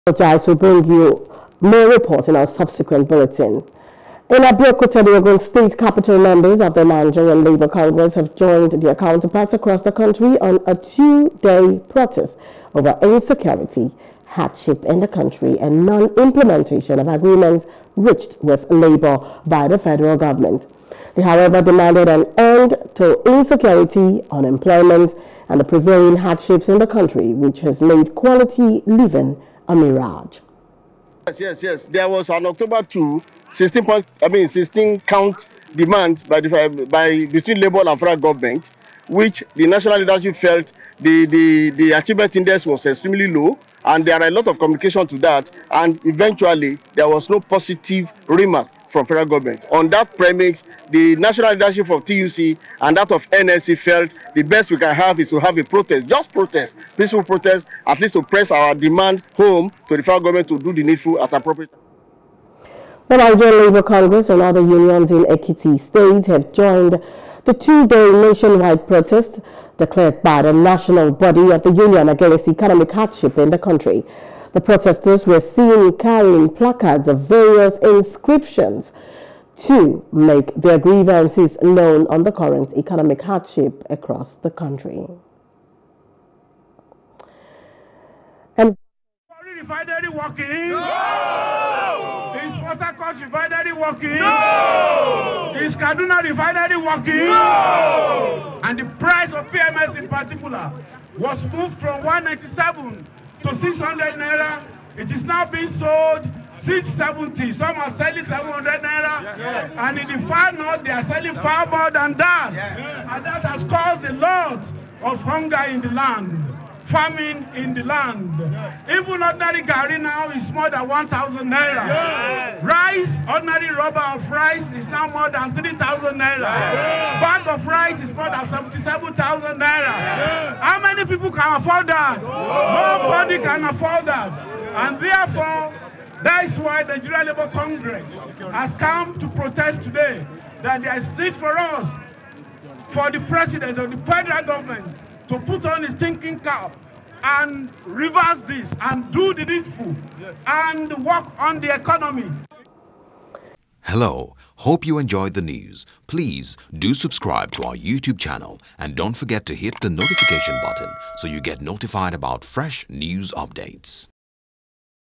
Ekiti State N L C, Other Unions Join Nationwide Protest Over Economic Hardship
The Nigeria Labour Congress (NLC) and other unions in Ekiti state have joined the two-day nationwide protest declared by the national body of the union against economic hardship in the country.